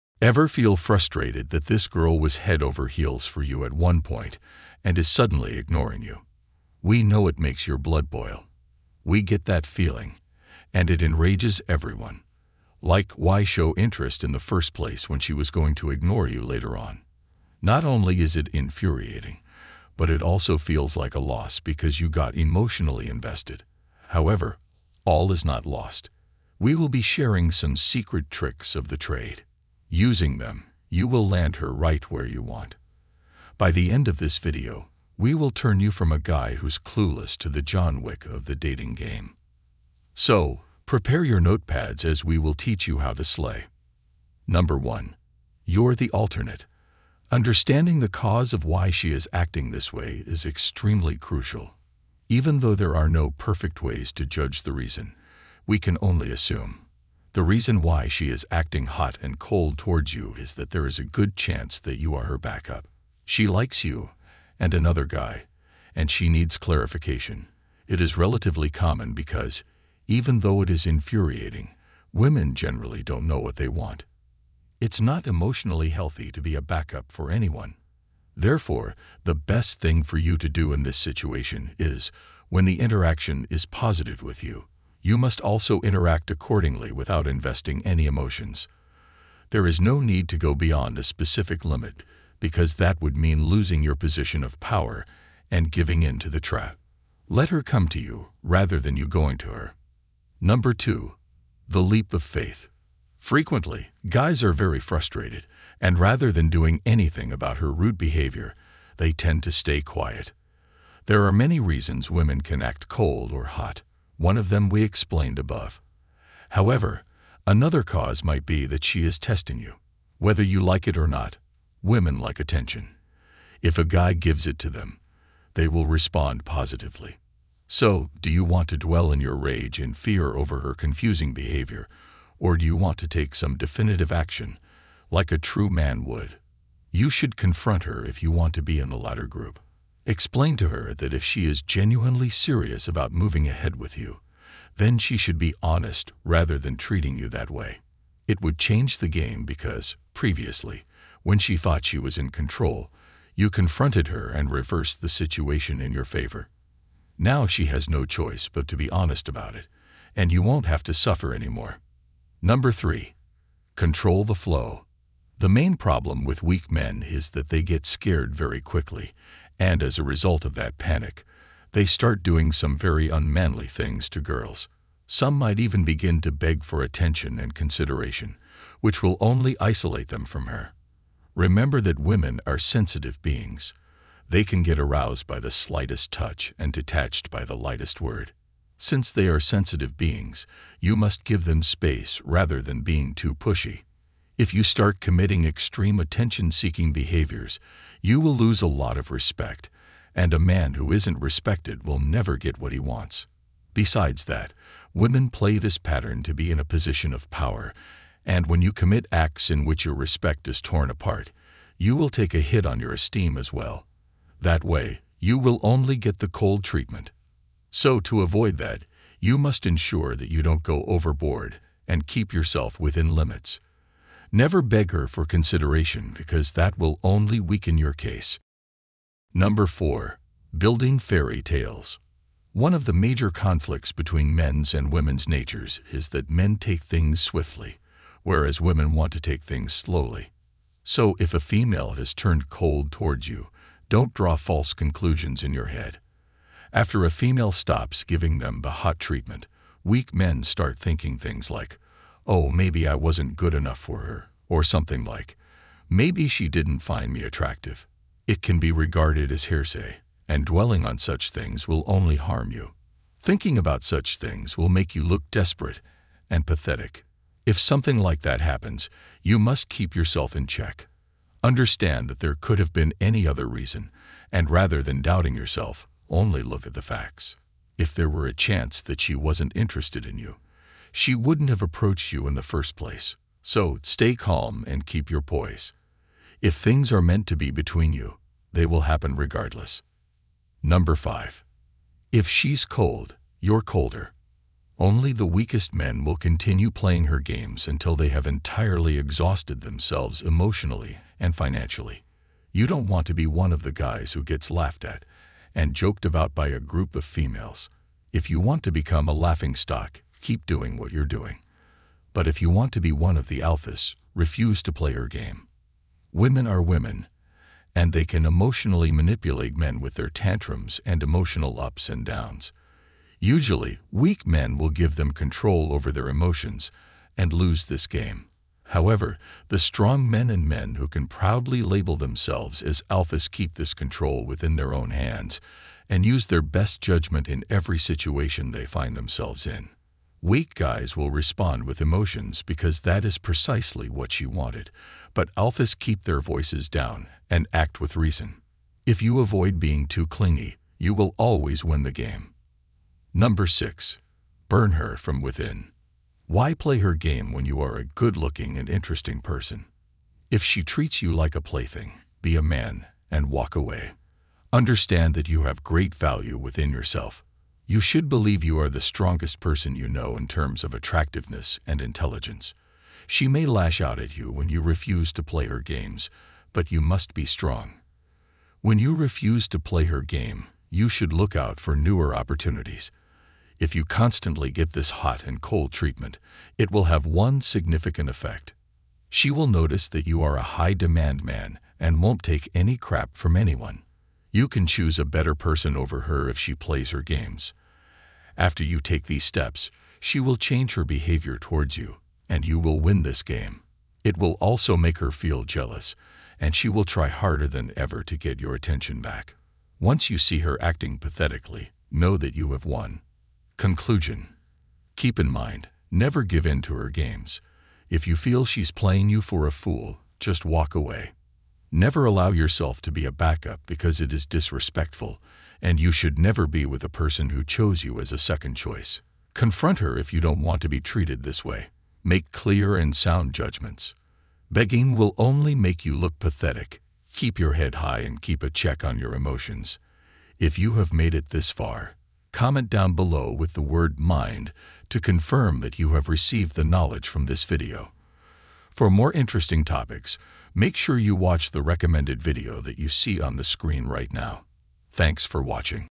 If She's Hot and Cold With You (Try This) | Mind Scraping
Are you dealing with a situation where someone you're interested in is sending mixed signals, being hot and cold? In this thought-provoking video from Mind Scraping, we explore strategies and insights to help you navigate these complex dynamics. Discover effective ways to decode the signals, communicate better, and gain a deeper understanding of what might be causing the hot-and-cold behavior. Don't let uncertainty in your relationships hold you back. Watch now and learn how to handle these situations with confidence and empathy.